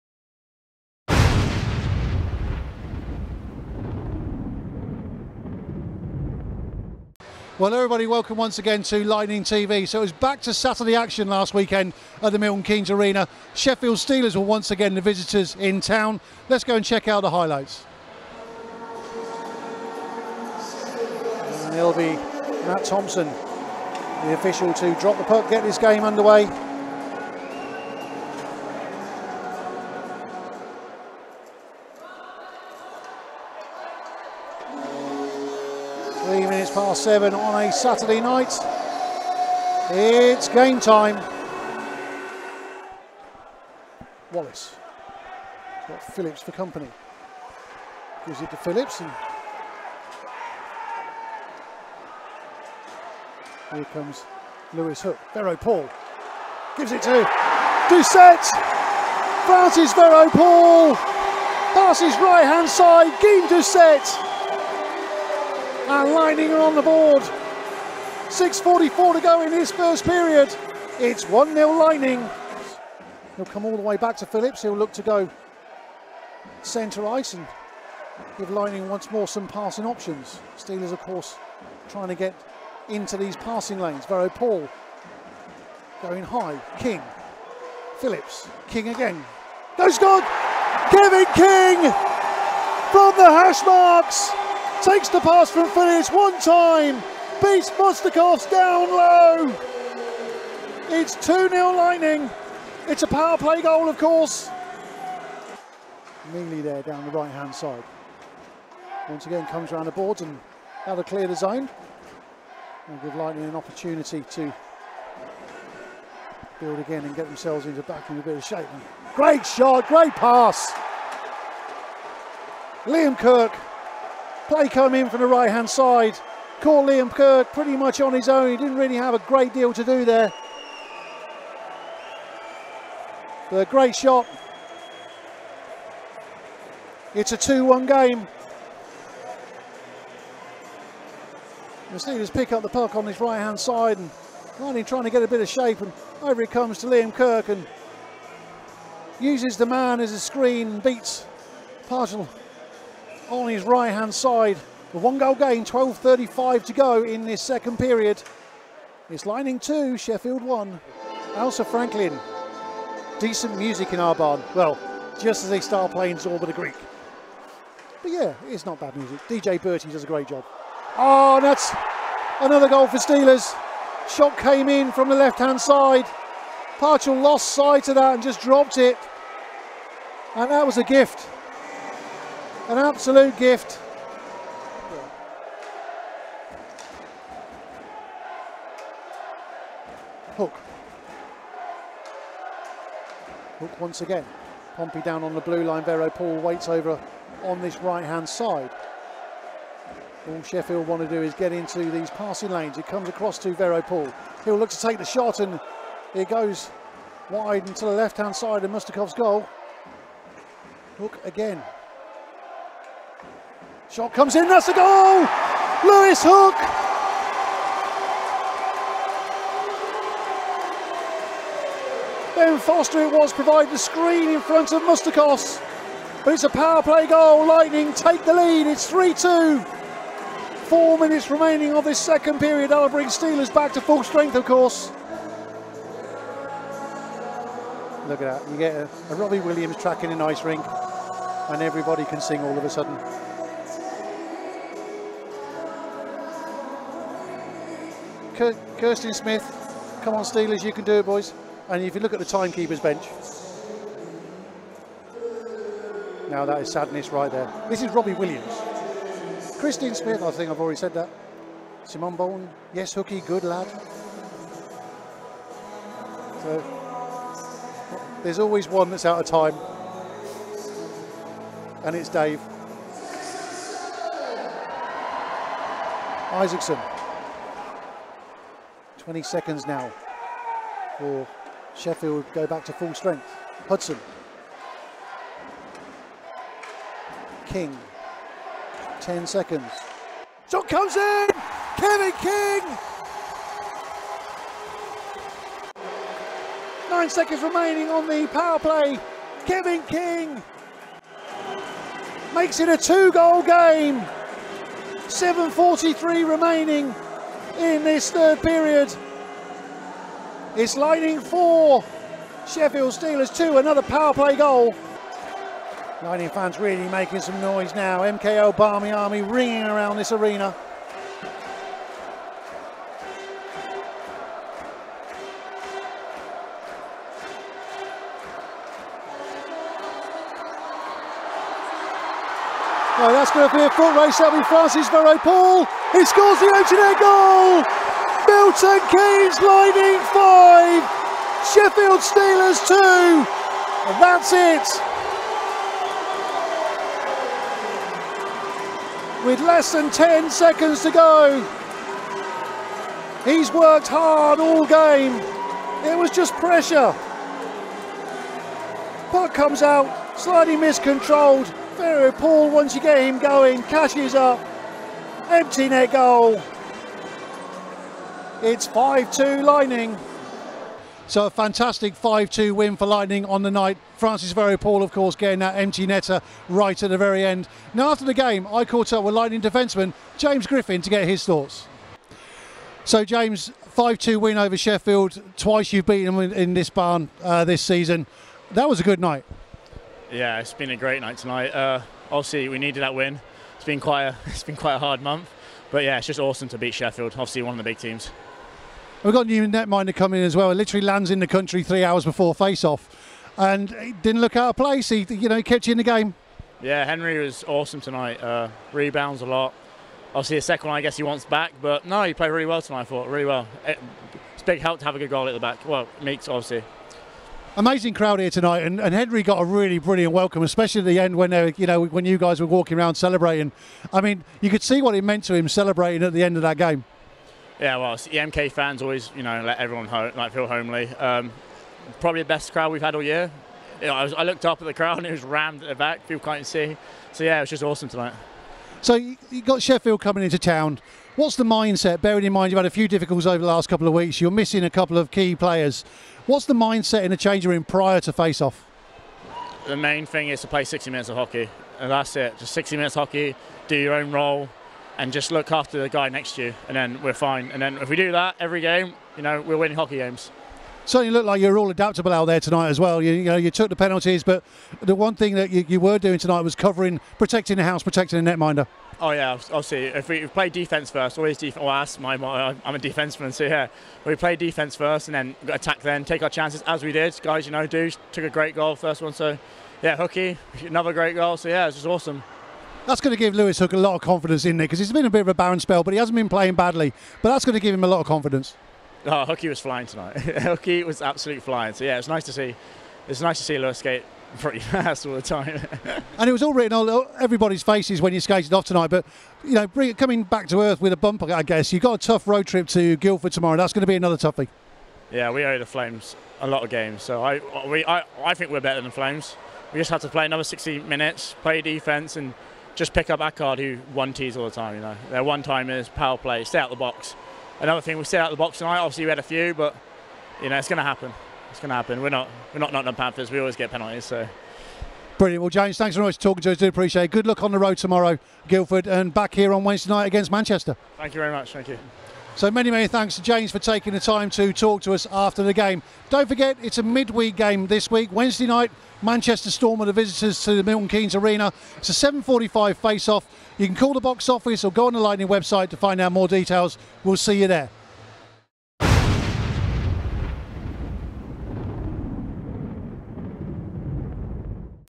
Lightning v Steelers 27th January 2018
Lightning host Sheffield in the Elite League Saturday 27th January 2018